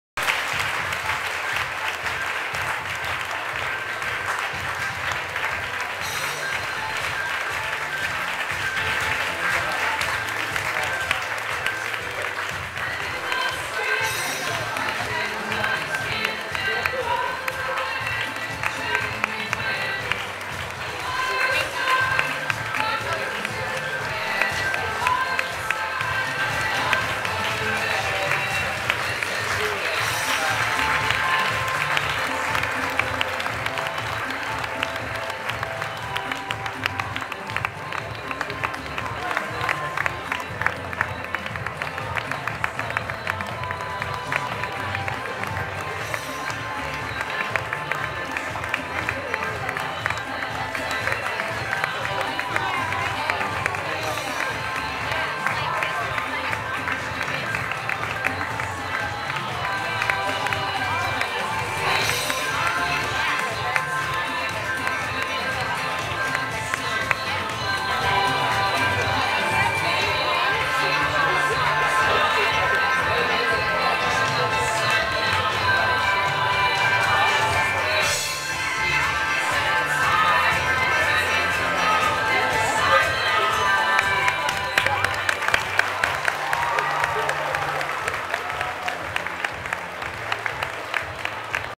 Thakoon Spring/Summer 2010
Finale of Thakoon's Spring/Summer 2010 Collection shown at Eyebeam Gallery during New York Fashion Week. Celebs and magazine editors were present of course, but Grace Coddington of Vogue got the most fans.